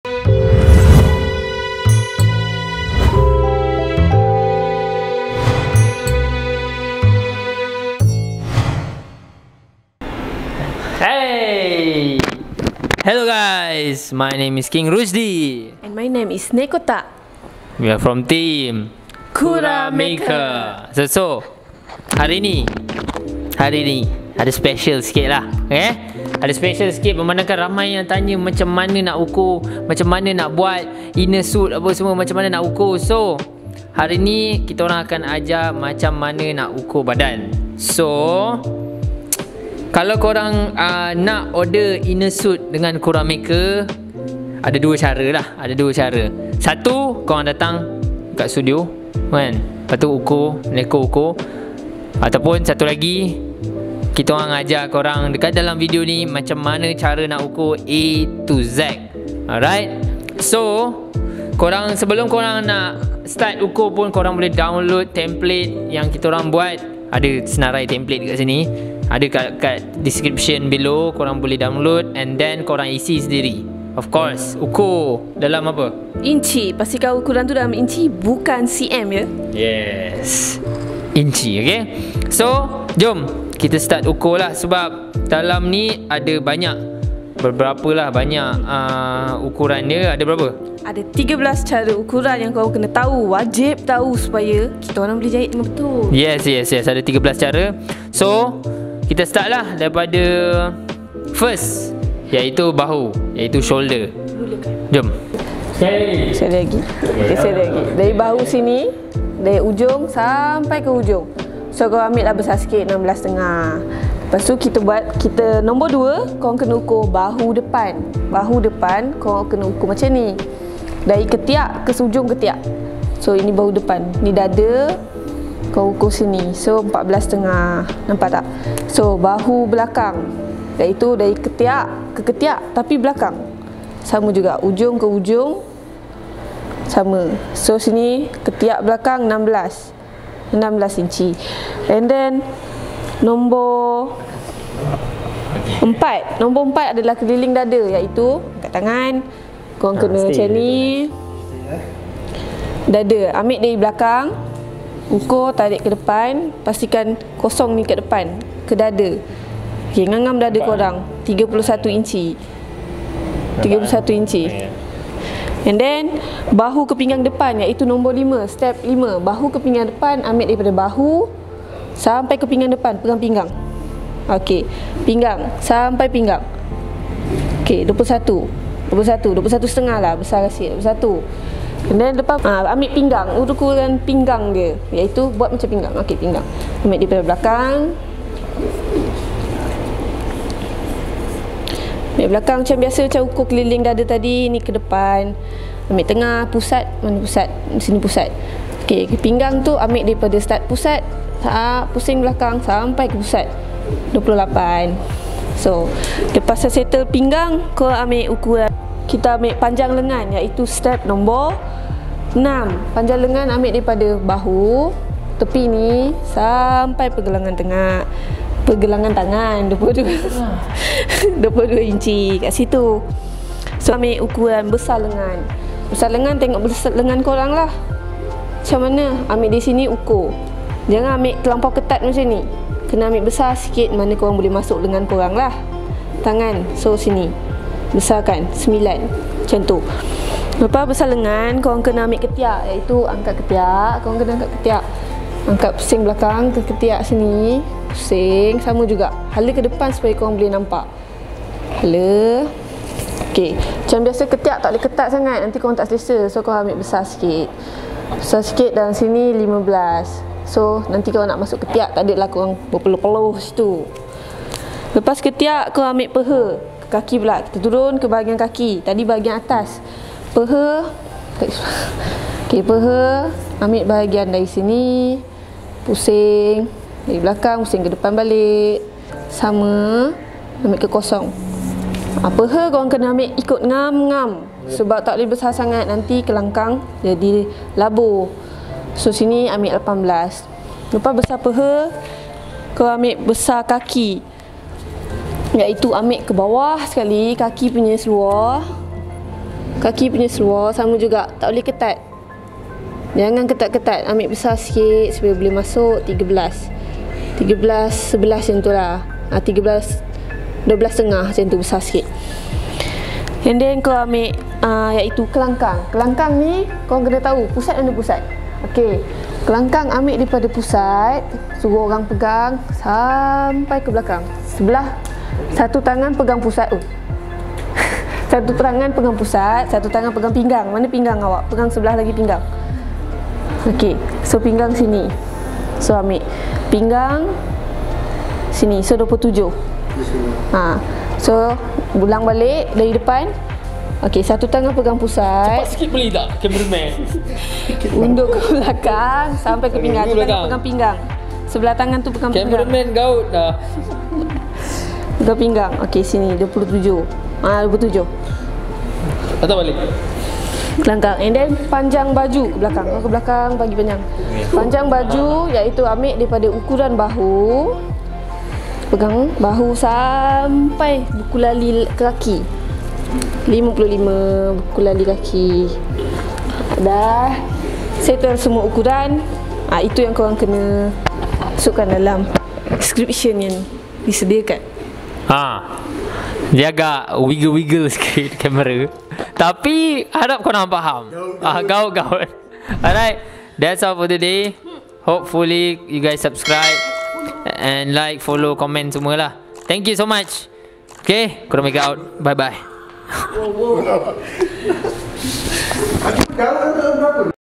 HOW TO : MEASURE YOUR INNER SUIT FOR COSPLAY (TOKUSATSU)
YOU GUYS ARE IN LUCK ! If you wanted to order inner suits (KAMEN RIDER OR SUPER SENTAI) with us ! here are HOW TO MEASURE IT !

PLEASE MEASURE IT USING INCH not CM okay !